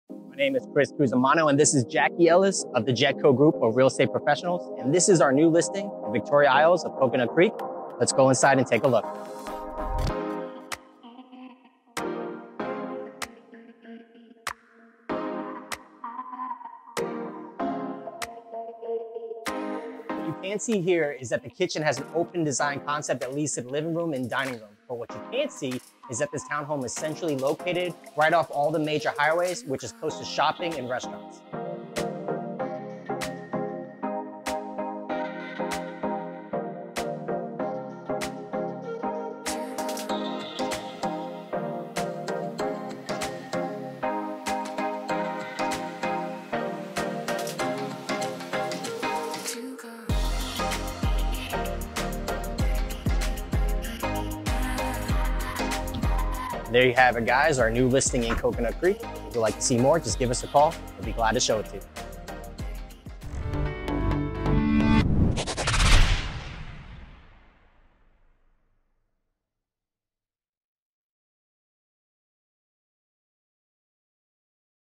Walkthrough Tour - 4750 NW 57th Place Coconut Creek, FL
When you're looking for townhomes you know that you want a corner unit. The additional light from the extra windows and the minimized noise from neighbors make these end units desirable and rare with only a handful of such townhomes coming on the market at any given time. Furthermore, the lack of back neighbors adds to your privacy and quiet location. Speaking of location, this is it. Located a stone's throw from the Sawgrass as well as endless dining and shopping, you won't be stressed about getting what you need done and going where you need to go. This location does this with ease. Victoria Isles allows two pets combined weight up to 50 lbs, is all ages, and family friendly complete with ample parking, community pool, sidewalks, landscaping & much more. Come look at your new home today!